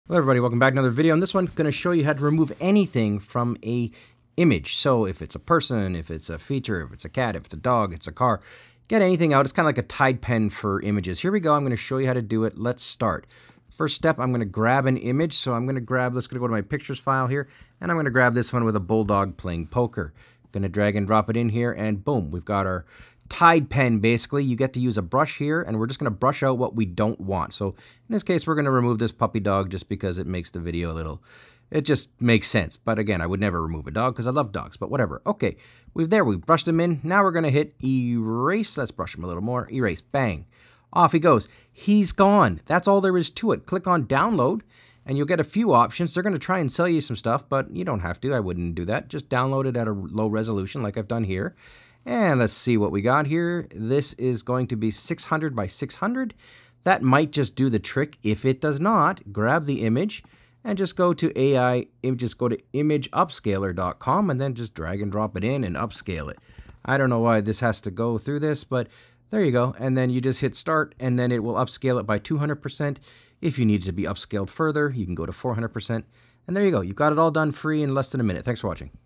How To Delete Anything From An Image - Free AI Tool Magic Studio
The speaker in the video is demonstrating how to remove an object from an image using an image editing tool. They start by importing an image and then use a brush tool to remove the object they don't want. After that, they use the erase function to finish removing the object. The speaker then mentions that they can download the edited image, but they advise against purchasing any additional options. Finally, they demonstrate how to upscale the image if needed, by using a separate tool.